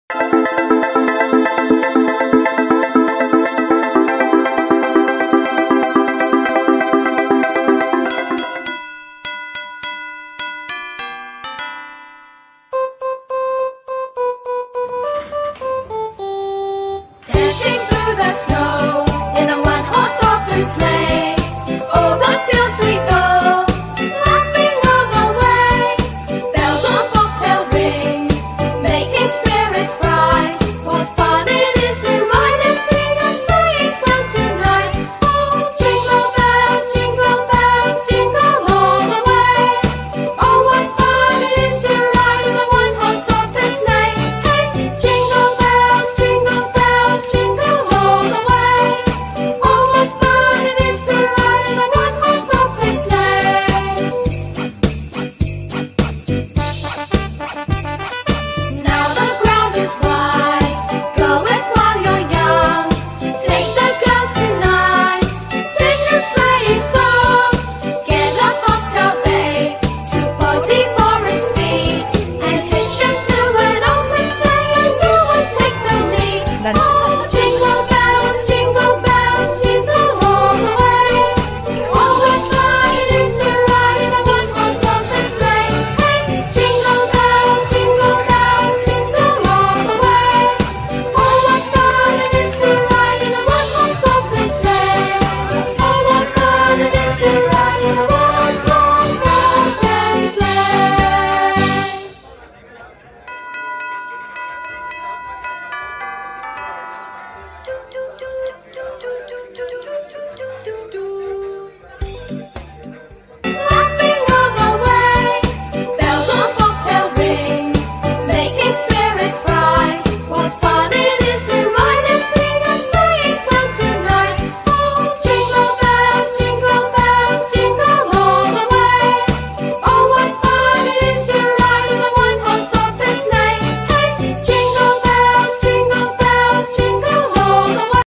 Christmas Party 2021 - Devex Hub
We, Devex Hub not only do the work all the time but also celebrate each festival with full energy and charisma.

Here, we captured the beautiful pictures of the Christmas party that we celebrated in our office. Hope everyone had a great Christmas evening with their friends, family members, and office colleagues.